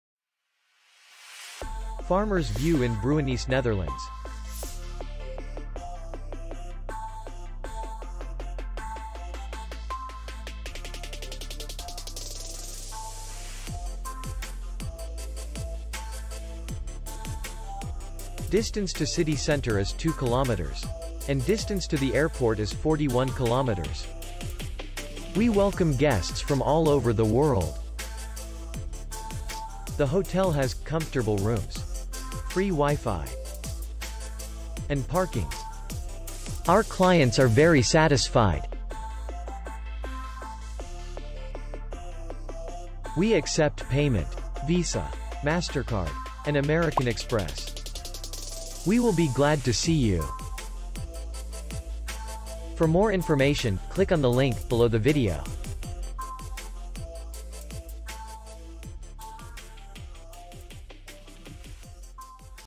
Farmer's View hotel review | Hotels in Bruinisse | Netherlands Hotels
Farmer's View hotel video review. Hotels in Bruinisse, european 3 stars hotels.